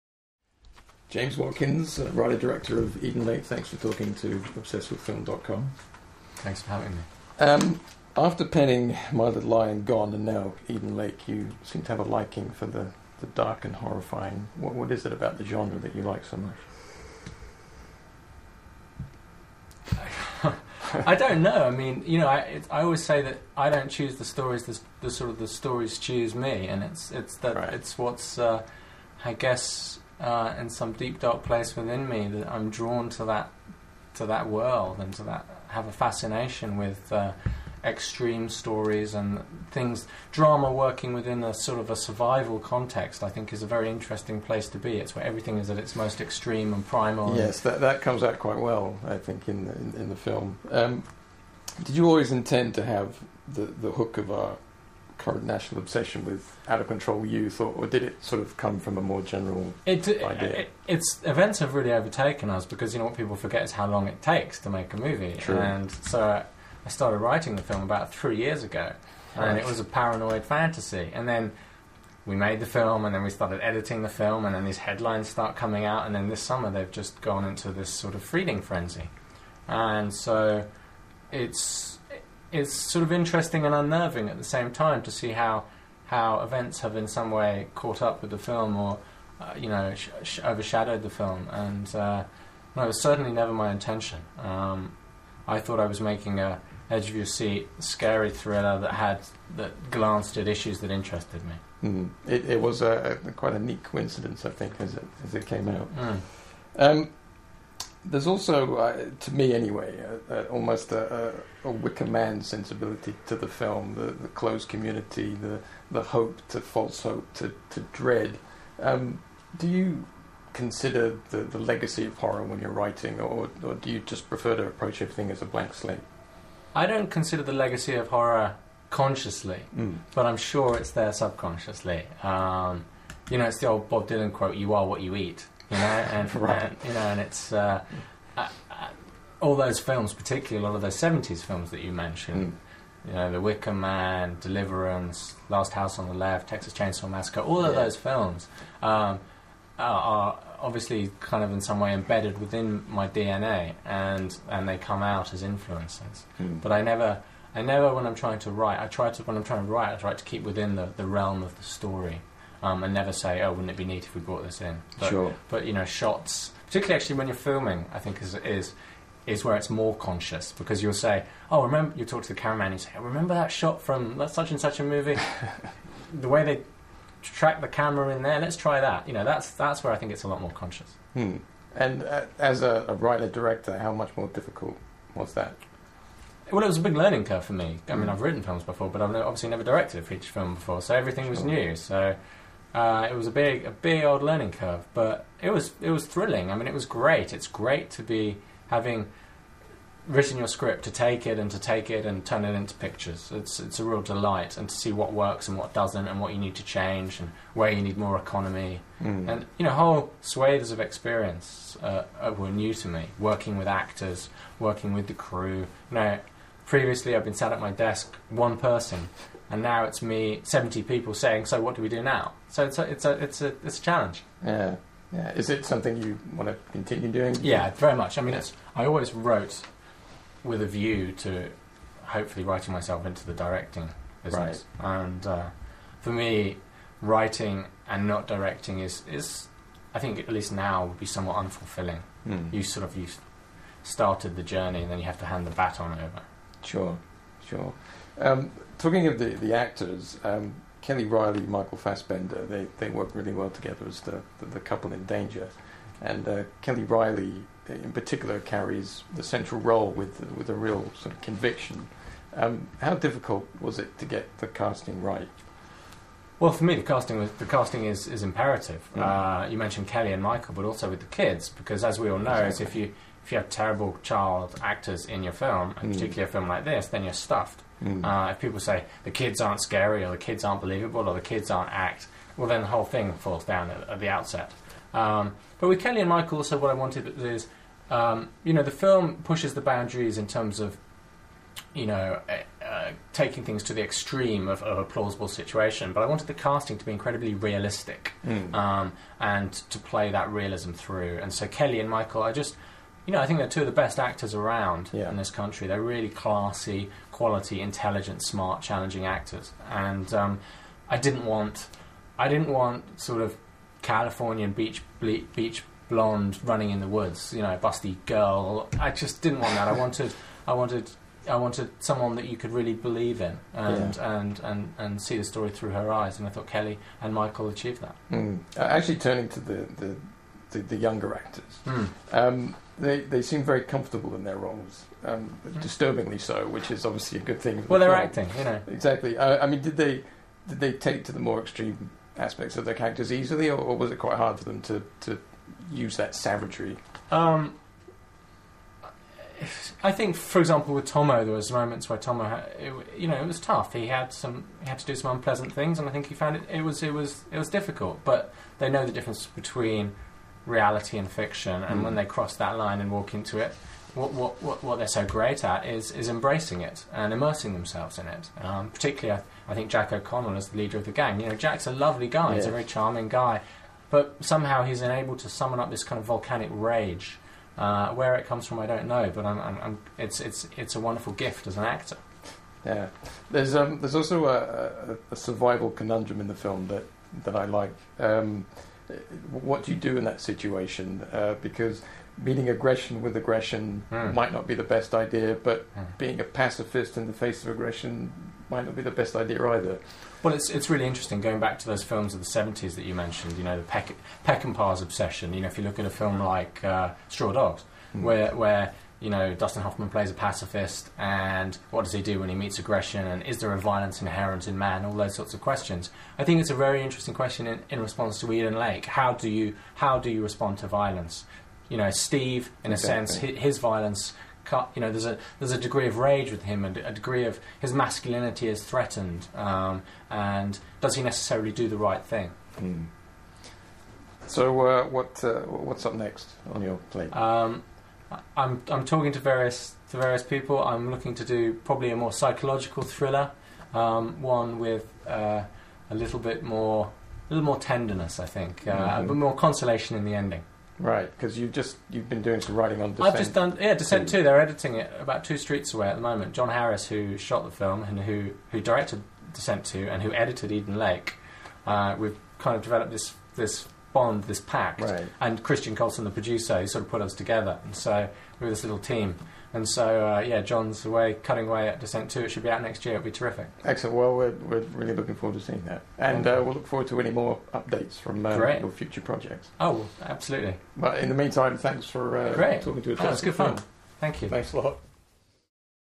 James Watkins, director of Eden Lake, interview
Obsessed With Film's Mark Clark catches up with James Watkins, director of Eden Lake.